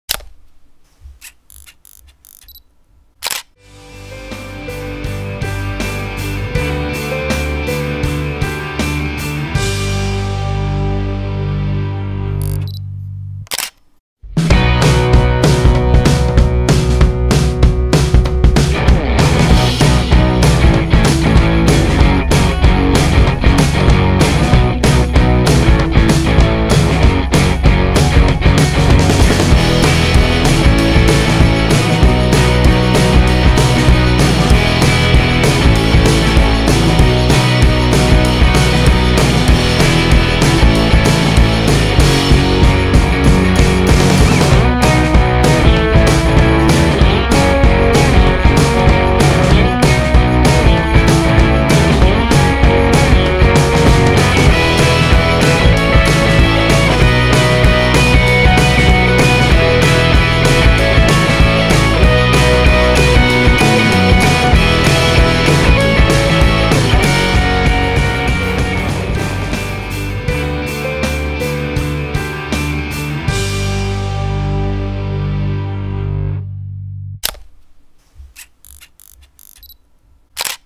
Fuji GFX Review - Coming Soon
We recently got our hands on the fantastic Fujifilm GFX.  We gave it a good test using it at both a wedding and at a model shoot.